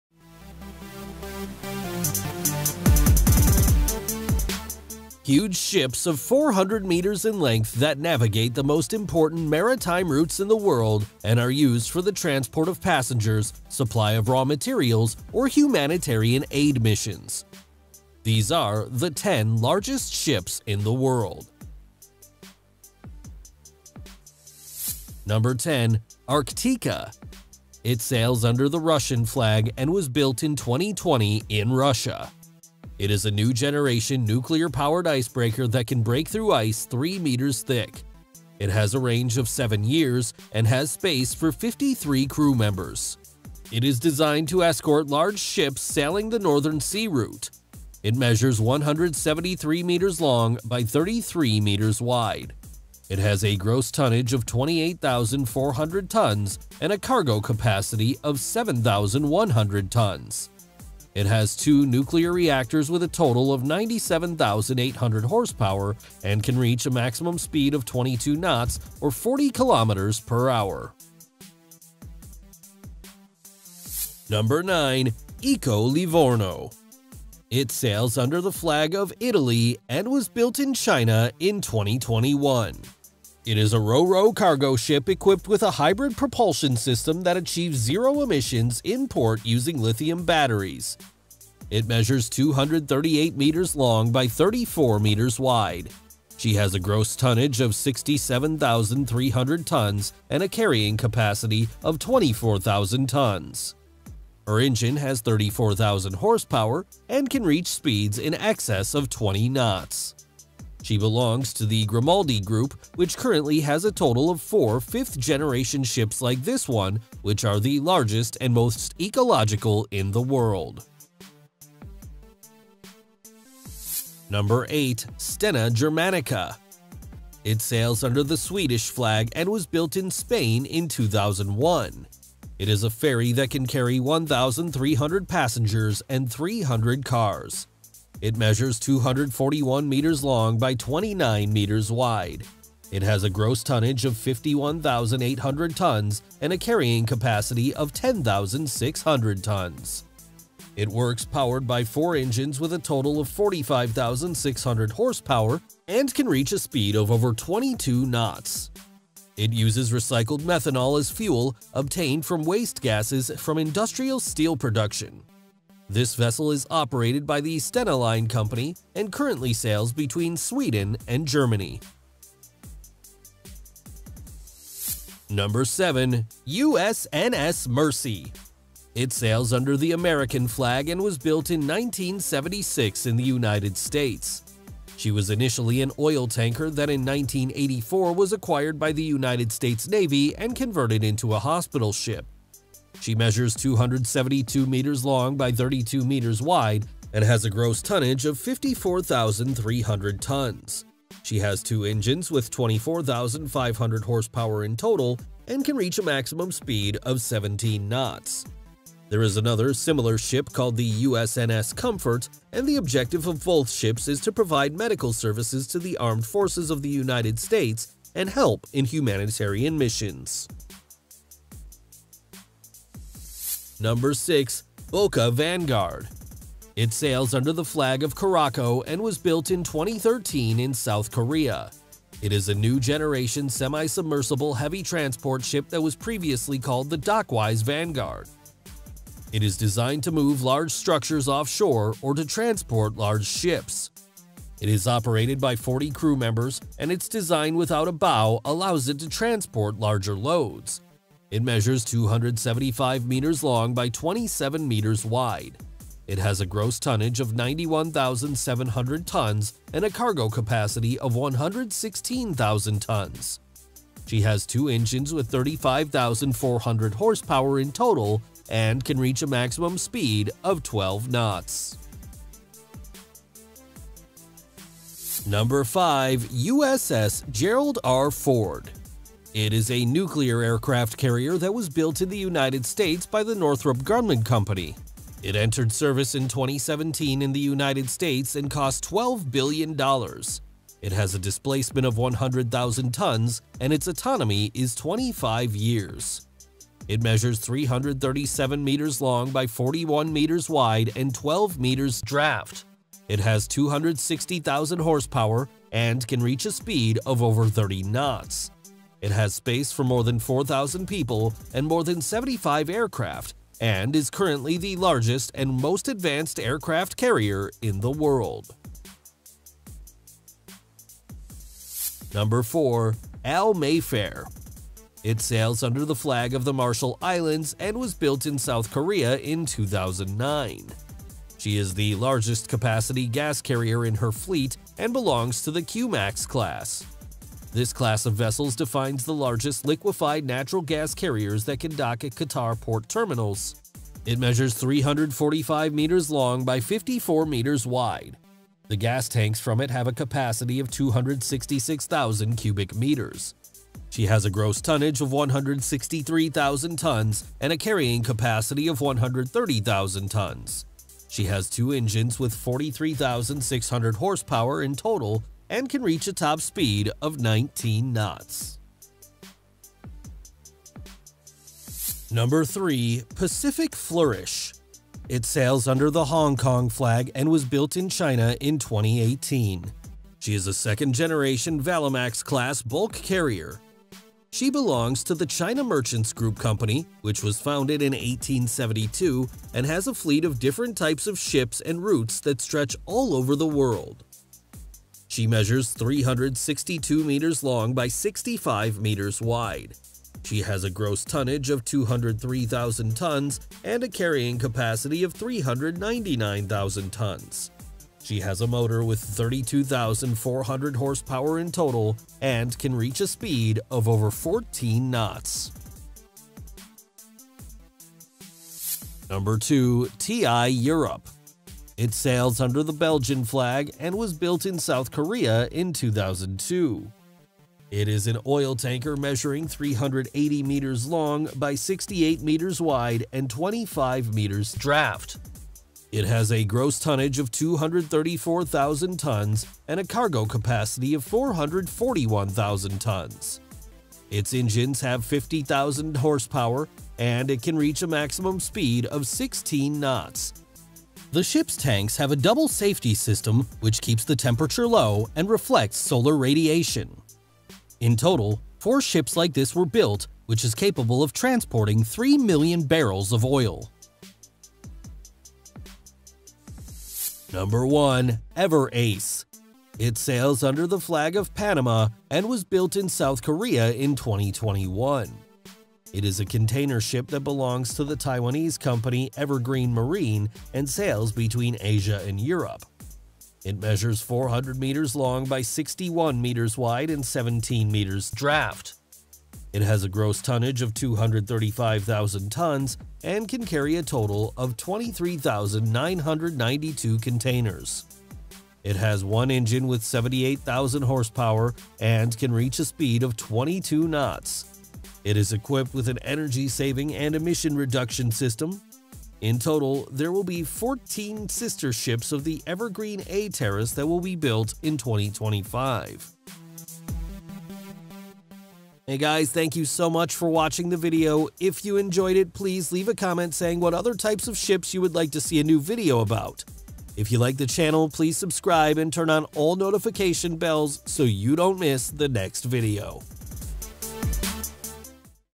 Dünyanın En Büyük 10 Gemisi 2023
Deniz taşımacılığı için dünyanın en büyük gemileri

Dünyanın en büyük gemileri, ana deniz yollarında seyreden, ham madde, yük veya konteyner gibi yolcu veya kargo taşımak için

yük gemileri ve konteyner gemileri, petrol tankerleri, dökme yük gemileri, uçak gemileri, buz kırıcılar, ro ro gemileri, metan gemileri, feribotlar, hastane gemileri veya yarı-dalgıç ağır nakliye gemileri gibi diğer gemi türlerinin ne olduğu hakkında teknik detaylar

00:00 Login
00:26 ARKTIKA
01:15 ECO LIVORNO
02:05 STENA GERMANICA
02:56 USNS Mercy
03:45 BOKA VANGUARD
04:37 USS Gerald R. Ford "CVN-78"
06:13 PACIFIC FLUORISH
07:48 EVER ACE
08:38 Exit